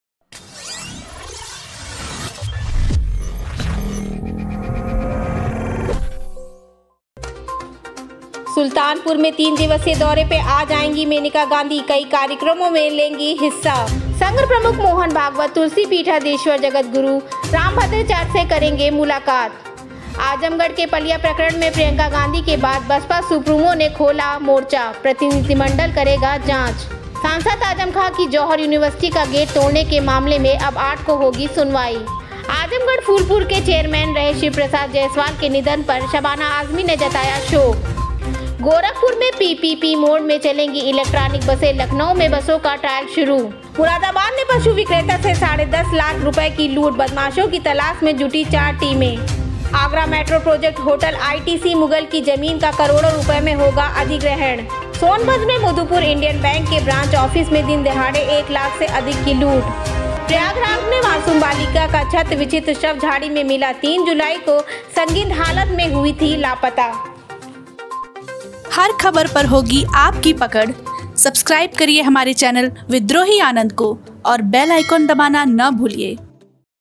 06 July 2021 Breaking News : UP से खास । Uttar Pradesh । Vidrohi ANAND।
1 सुल्तानपुर में तीन दिवसीय दौरे पर आज आएंगी मेनका गांधी

2 संघ प्रमुख मोहन भागवत जगद्गुरु रामभद्राचार्य से करेंगे मुलाकात

3 आजमगढ़ के पलिया प्रकरण में बसपा सुप्रीमो ने खोला मोर्चा

4 जौहर यूनिवर्सिटी का गेट तोड़ने के मामले में 8 को होगी सुनवाई

5 शिवप्रसाद जायसवाल के निधन पर शबाना आजमी ने जताया शोक

6 गोरखपुर में पीपीपी मोड में चलेंगी इलेक्ट्रिक बसें

7 मुरादाबाद में पशु विक्रेता से साढ़े 10 लाख रुपये की लूट

8 आगरा मेट्रो प्रोजेक्ट

9 इंडियन बैंक के ब्रांच ऑफिस में दिनदहाड़े एक लाख से अधिक की लूट

10 प्रयागराज में मासूम बालिका का क्षत-विक्षत शव झाड़ी में मिला